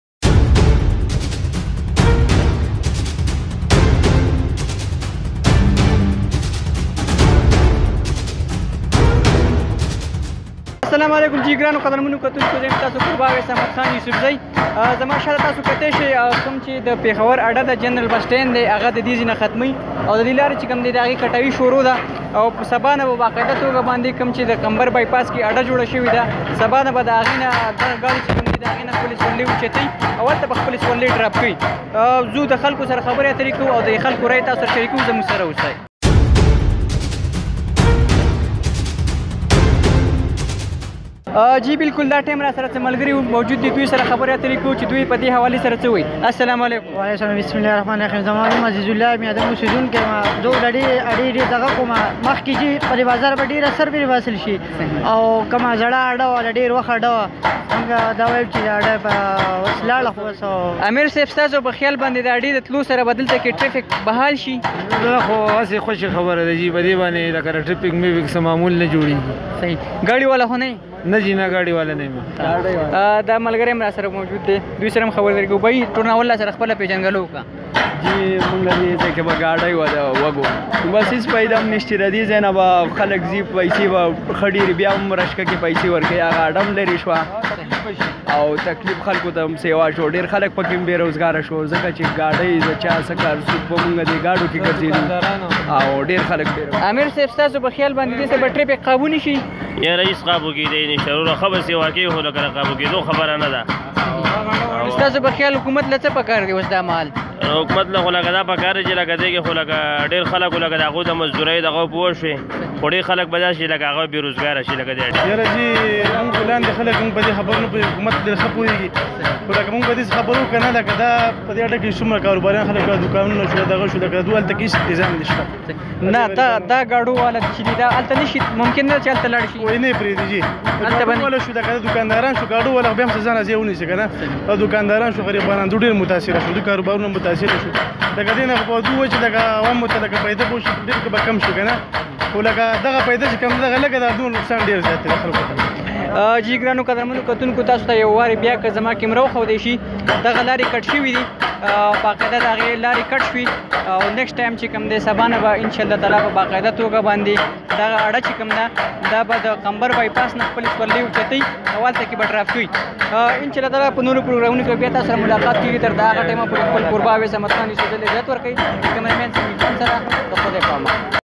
سوات، مینگورہ جنرل بس سٹینڈ کی منتقلی کی ڈیڈلائن مکمل
سوات، مینگورہ جنرل بس سٹینڈ کی منتقلی کی ڈیڈلائن مکمل ، آڈہ سیل ، ، داخلی اور خارجی راستون کی کھدائی ، مزید دیکھئے اویس احمد یوسفزئی کی اس رپورٹ میں